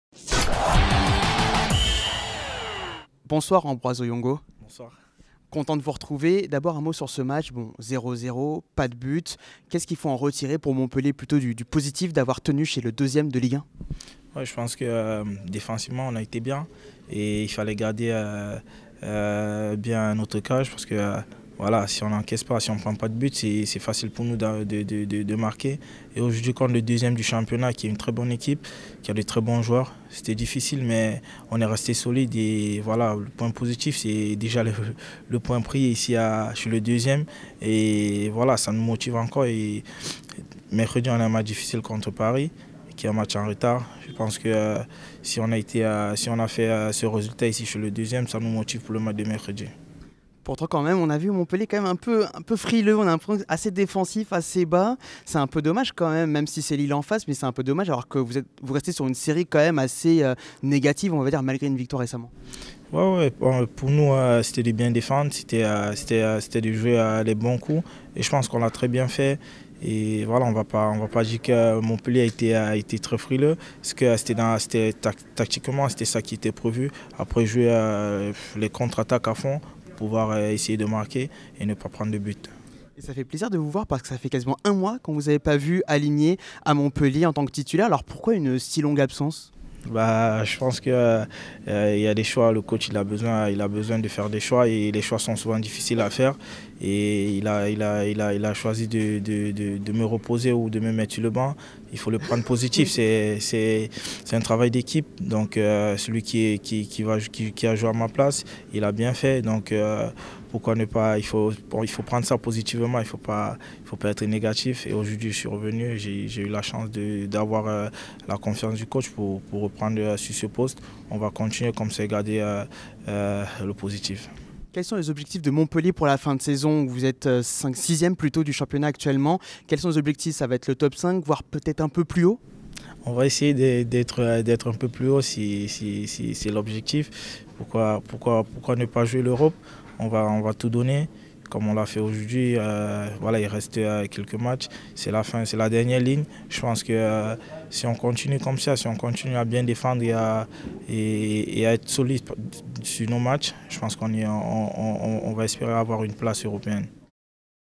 "Il faut rester positif" : A. Oyongo (Montpellier) revient après un mois sur le banc
A l'issue d'un terne match Lille-Montpellier (0-0, Ligue 1, 25è journée), Ambroise Oyongo s'est arrêté à notre micro.

Dans cette première partie d'interview, le latéral camerounais souligne la belle résistance collective des héraultais, mais aussi les objectifs pour cette fin de saison.

Relégué sur le banc des remplaçants pendant un mois, Oyongo revenait dans le onze titulaire au Stade Pierre-Mauroy dimanche. Pourquoi un si longue éclipse? Il répond.

Interview réalisée par Karim Baldé et Louis Decroix pour TV5 Monde.